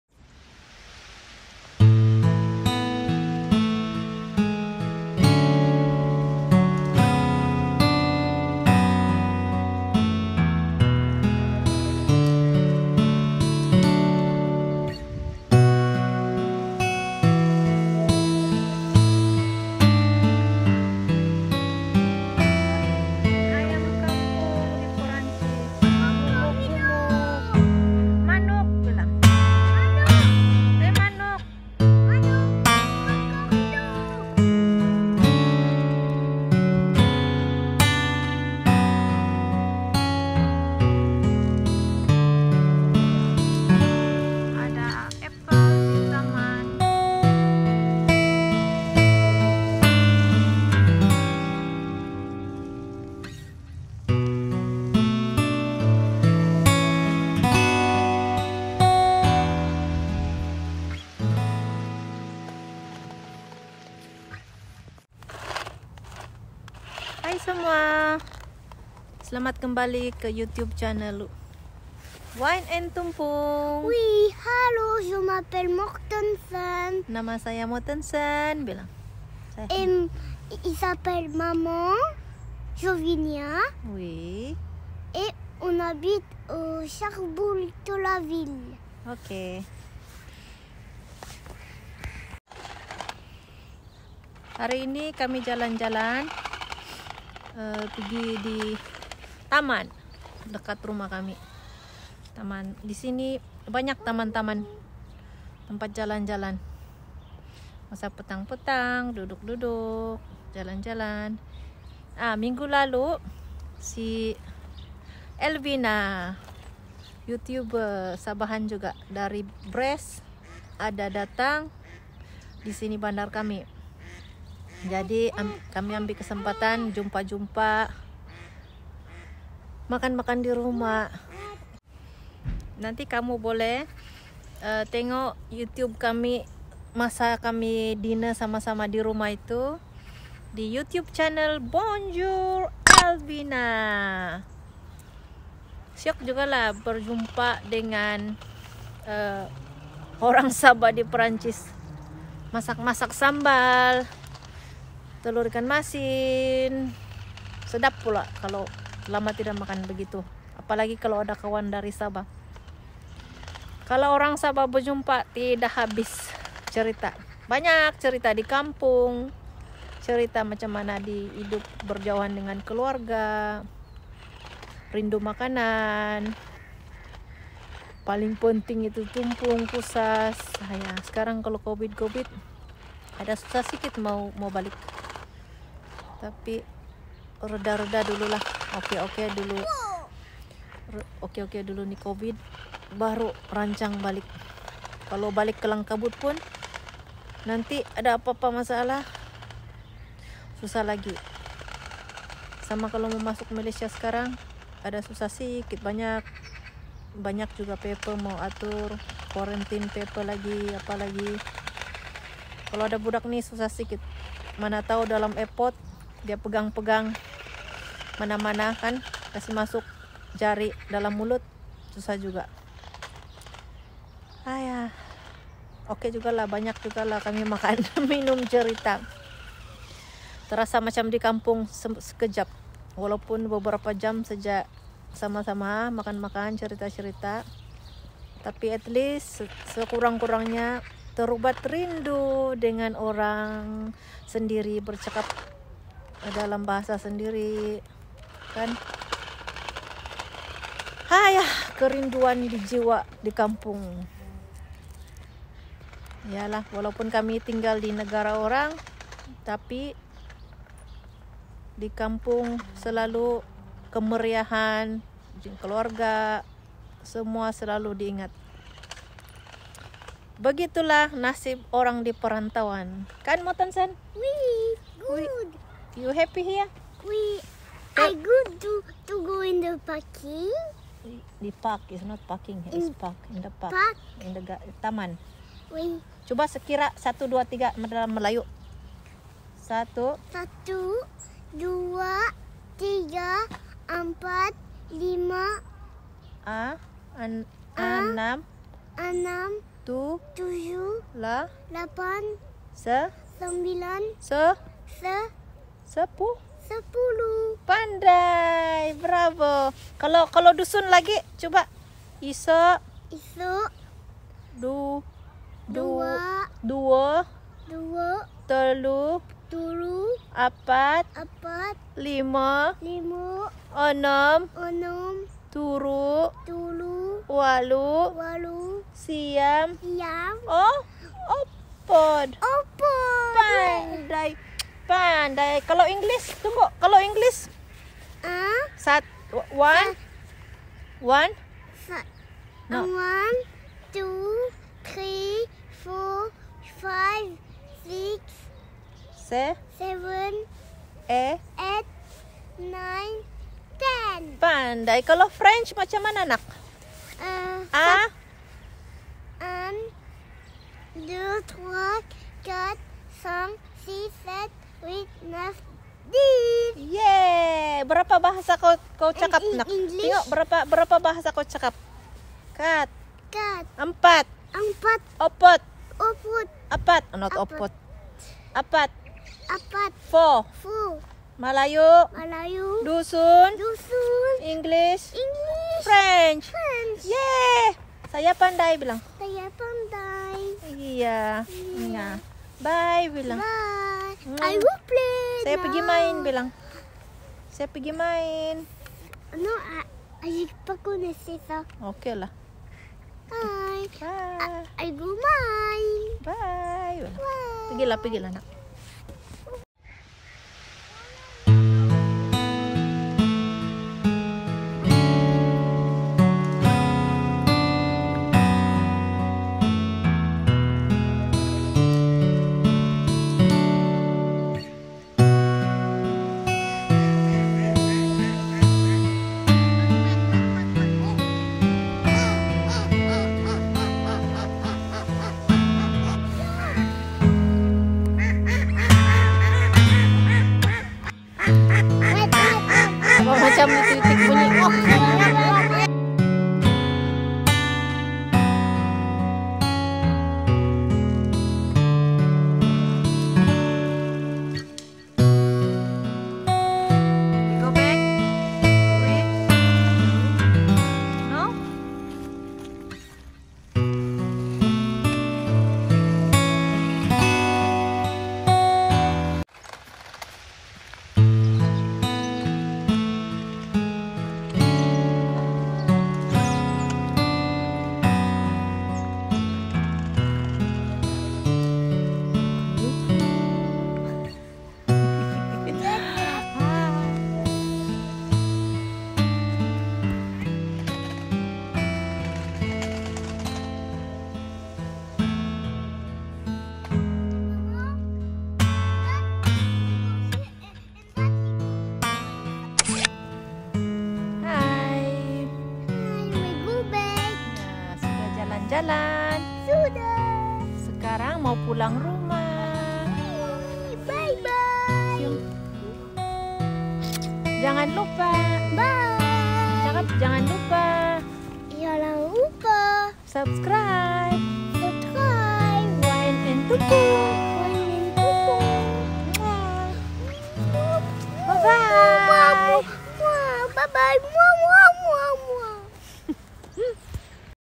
Rindu Keluarga ❤️ Jalan-Jalan Di Taman Semulajadi
Bila teringatkan kampung halaman dan keluarga yang jauh di tinggalkan Selalu nya saya akan berjalan jaln di mana2 Taman berdekatan dengan kawasan rumah... atau menyibuk kan diri dengan activity2 riadah atau membawa anak jalan2